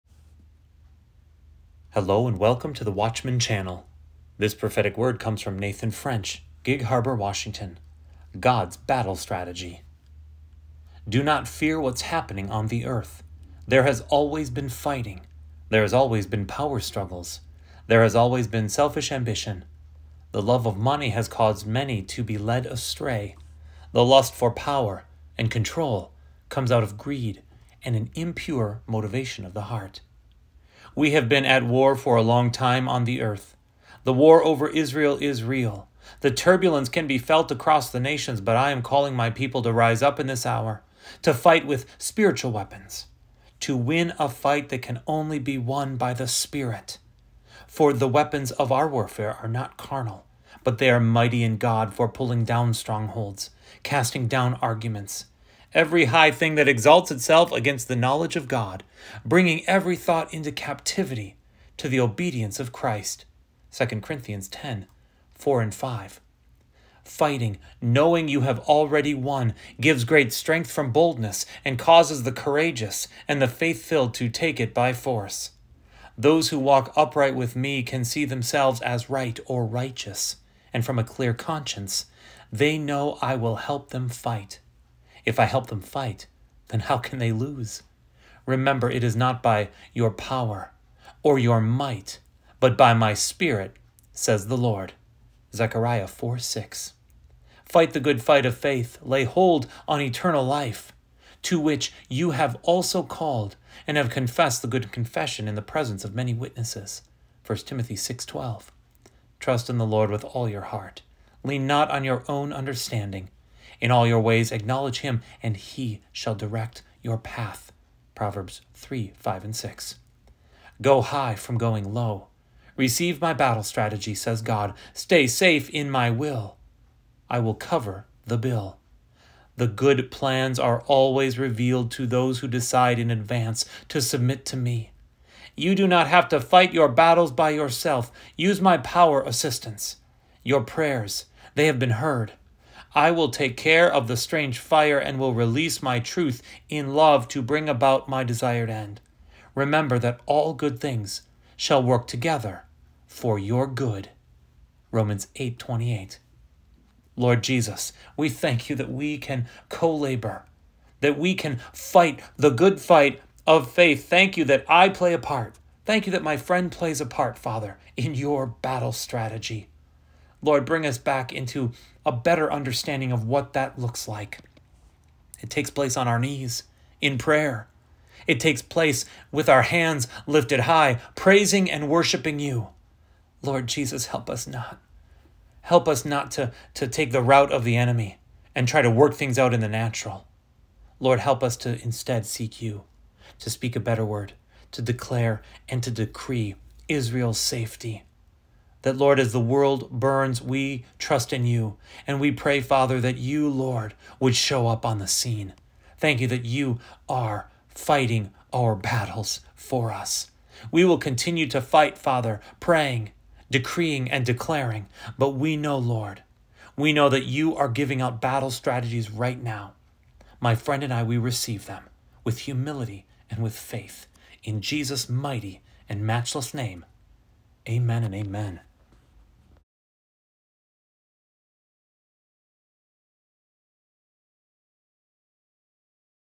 “God’s Battle Strategy” – Powerful Prophetic Encouragement from Nathan French
Powerful Original Word Released By Nathan French (Gig Harbor, WA) on October 13, 2023 with prayer & commentary from Watchmen on the Wall